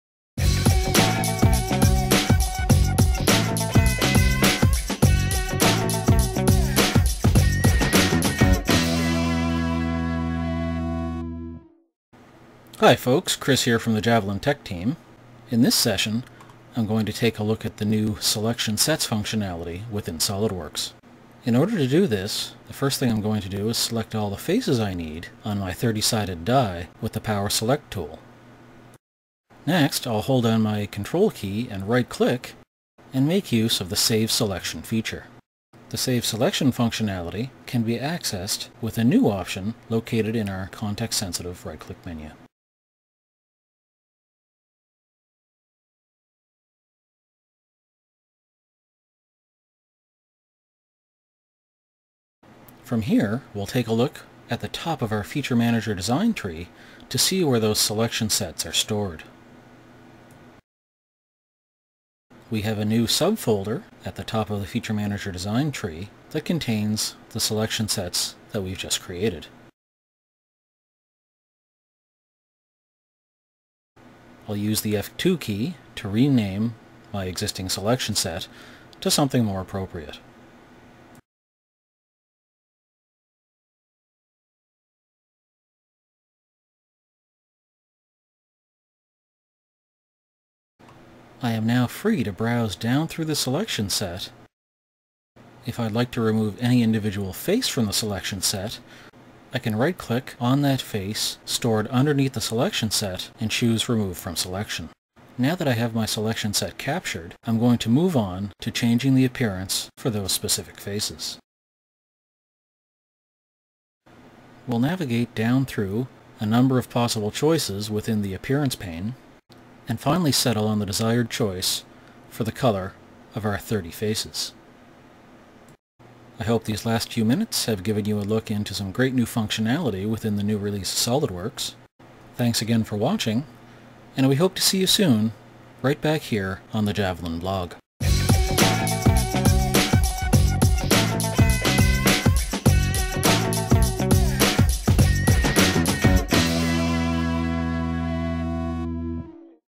Tech Tip: SOLIDWORKS 2015 Tutorial - Saved Selection Sets
Selection Sets enable the capture of important information, reducing repetitive selection operations during modelling, assembly, or simulation setup.

Selection Sets are saved in the SOLIDWORKS file and are accessed from the FeatureManager. They can include geometry, components, or drawing items.